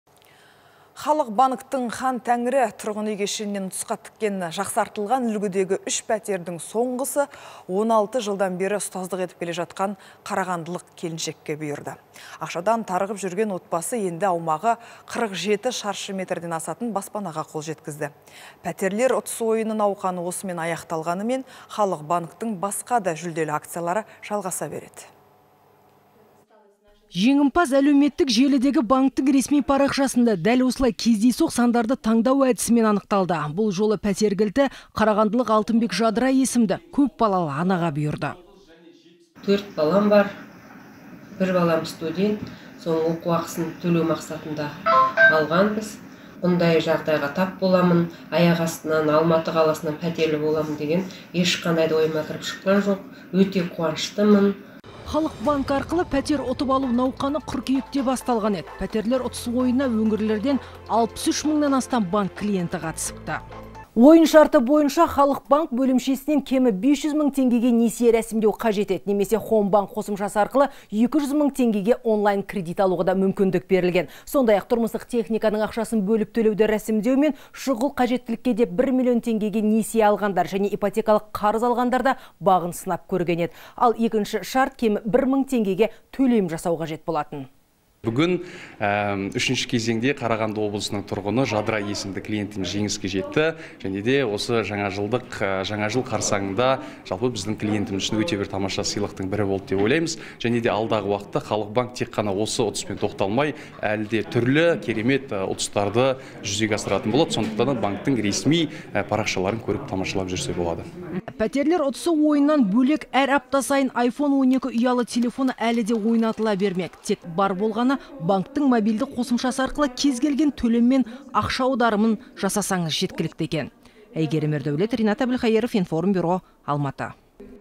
«Halyk Bank» ұтысқа тіккен кезекті пәтер қарағандылық келіншекке бұйырды
Халық банктің «Хан Тәңірі» тұрғын үй кешенінен ұтысқа тіккен жақсартылған үлгідегі 3 пәтердің соңғысы 16 жылдан бері ұстаздық етіп келе жатқан қарағандылық келіншекке бұйырды. Ақшадан тарығып жүрген отбасы енді аумағы 47 шаршы метрден асатын баспанаға қол жеткізді. «Пәтерлер ұтыс ойыны» науқаны осымен аяқталғанымен, Халық банктің басқа да жүлделі акциялары жалғаса береді.
Жеңімпаз әлеуметтік желідегі банктің ресми парақшасында, дәл осылай кездейсоқ сандарды таңдау әдісімен анықталды. Бұл жолы пәтер кілті қарағандылық Алтынбек Жадыра есімді көпбалалы анаға бұйырды.
ЖАДЫРА АЛТЫНБЕК, жеңімпаз:
– 4 балам бар. Бір балам студент. Сол оқу ақысын төлеу мақсатында алғанбыз. Бүгінгі күнде енді аяқ-астынан Халық банкінің пәтер ойнатып жатқанын естігенмін. Несие алғанда ескертіліп айтылған. Бірақ Алматыдан пәтерлі боламын деп ойлаған жоқпын, өте қуаныштымын!
«Halyk Bank» арқылы пәтер ұтып алу науқаны қыркүйекте басталған еді. «Пәтерлер ұтыс ойынына» өңірлерден 63 мыңнан астам банк клиенті қатысыпты.
ӘЙГЕРІМ ЕРДӘУЛЕТҚЫЗЫ, тілші:
– Ойын шарты бойынша «Halyk Bank» бөлімшесінен кемі 500 мың теңгеге несие рәсімдеу қажет еді. Немесе "Homebank" қосымшасы арқылы 200 мың теңге онлайн-кредит алуға да мүмкіндік берілген. Сондай-ақ, тұрмыстық техниканың ақшасын бөліп төлеуді рәсімдеу мен шұғыл қажеттілікке деп 1 миллион теңге несие алғандар және ипотекалық қарыз алғандар бағын сынап көрген еді. Ал екінші шарт- кемі 1 мың теңгеге төлем жасау қажет болатын.
ДОСБОЛ АРЫСТАНБЕКОВ, «Қазақстан Халық Банкі» АҚ PR-менеджері:
– Үшінші кезеңде Қарағанды облысының тұрғыны Жадыра есімді  клиентіміз жеңіске жетті және жаңа жыл қарсаңында өте керемет сыйлықтың бірі болды деп ойлаймыз. Алдағы уақытта Халық банк осы ұтыспен тоқталмай, әлі де кермет ұтыстарды жүзеге асыратын болады. Сондықтан да банктің ресми парақшаларын көріп тамашалап жүрсе болады.
Иә, "пәтерлер ұстысы ойынынан" бөлек әр апта сайын Айфон 12 ұялы телефоны әлі де ойнатыла бермек. Тек бар болғаны, банктің мобильді қосымшасы арқылы кез-келген төлем мен ақша аударымын жасаңыз жеткілікті екен.